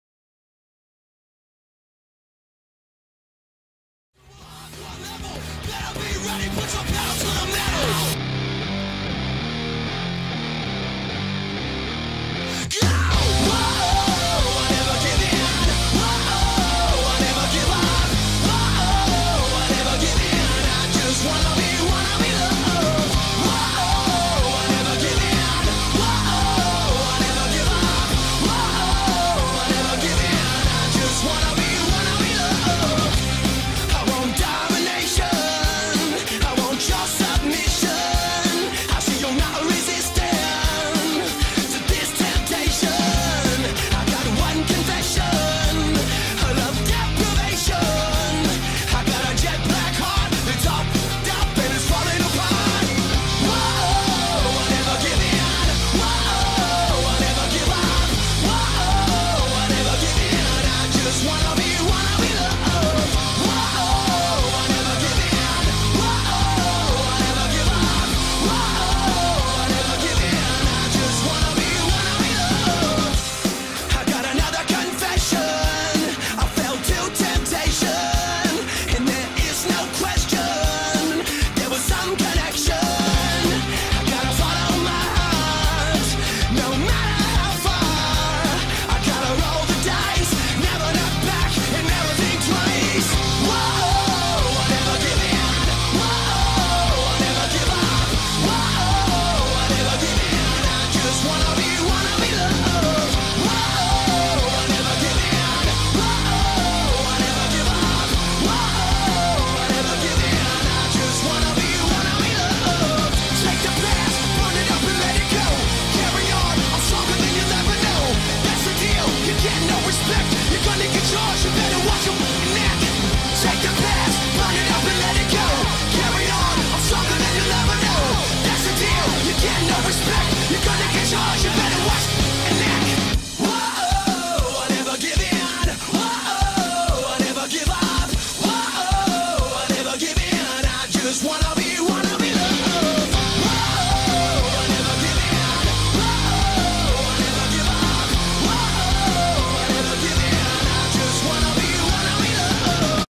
Easton U18 Ice Hockey
montage of u18 travel ice hawks, preseason